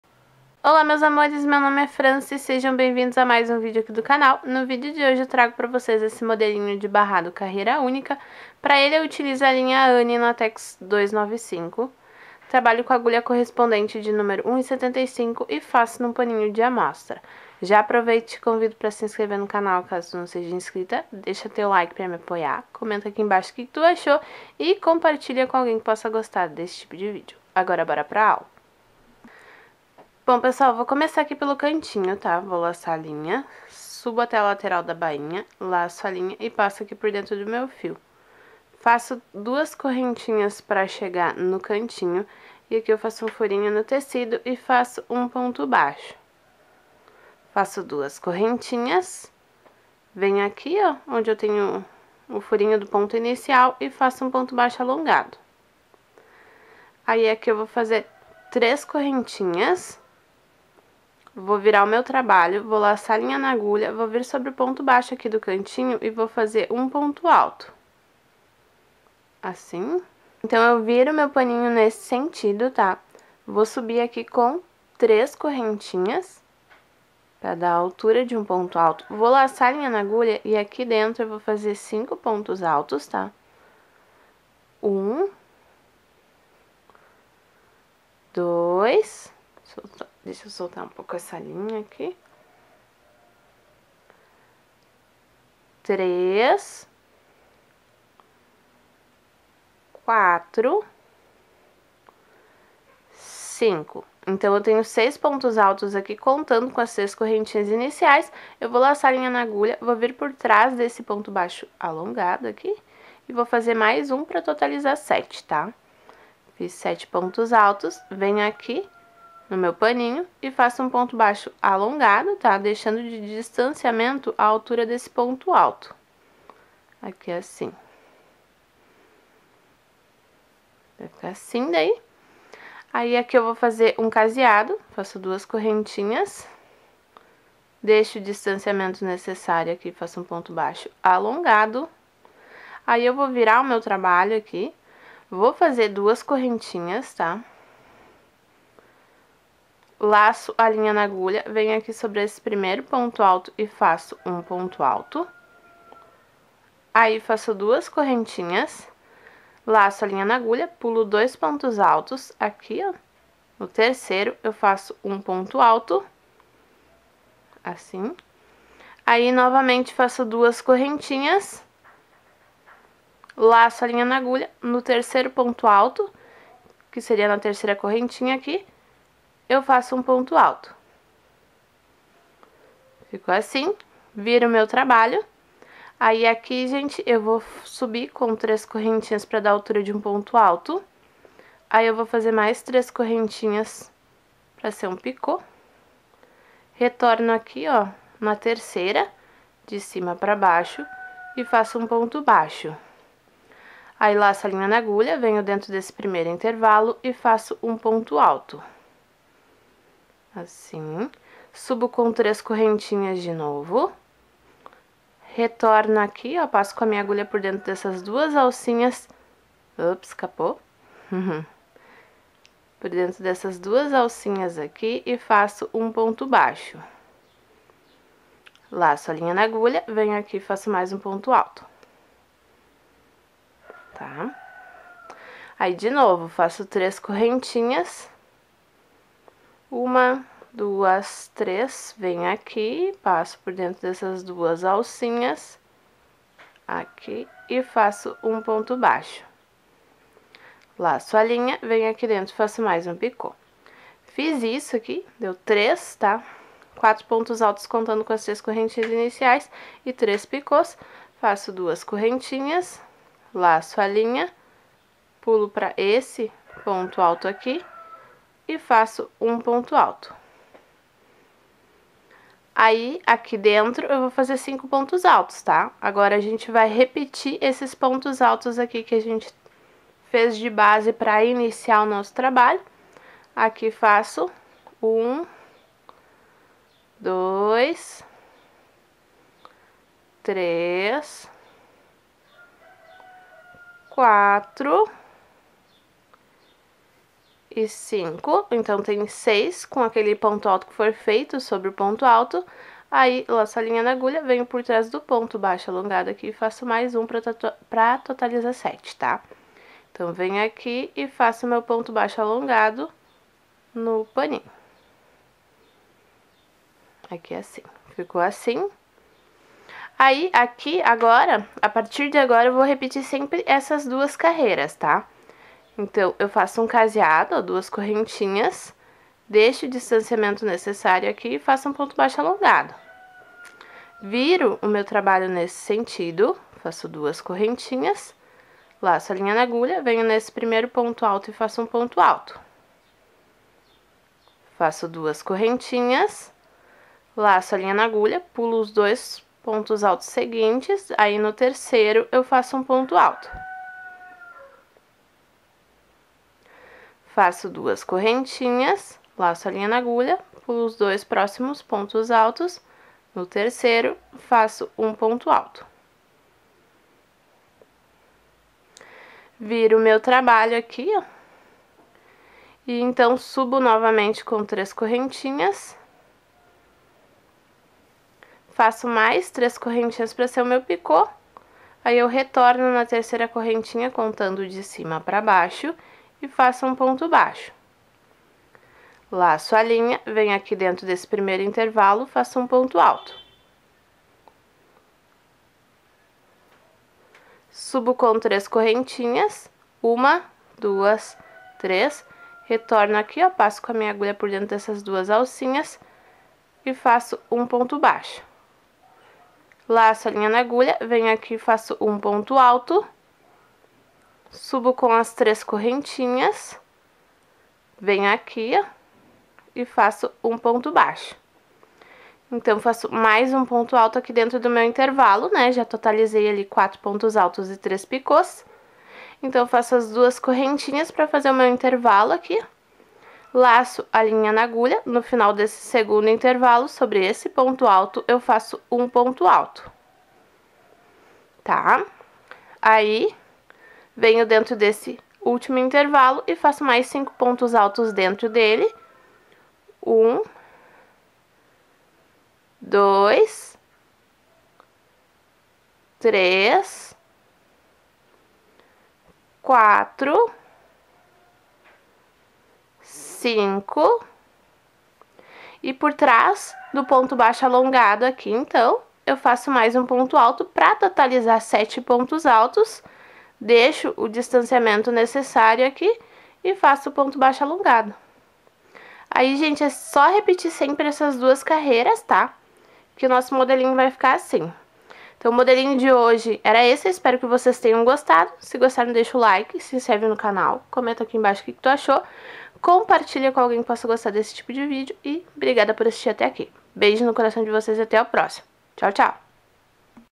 BARRADO DE CROCHÊ SUPER FÁCIL CARREIRA ÚNICA VERSÃO PARA CANHOTO #483 | BICO DE CROCHÊ
No vídeo de hoje compartilho mais um passo a passo de barrado de crochê versão para canhoto, modelo simples, fácil de fazer e rápido, ótimo para fazer em pano de prato, toalha de mesa, toalha de banho e onde você desejar. Confira!

Material utilizado:

- Linha anne;
- Agulha 1,75 mm;
- Pano de prato;
- Tesoura.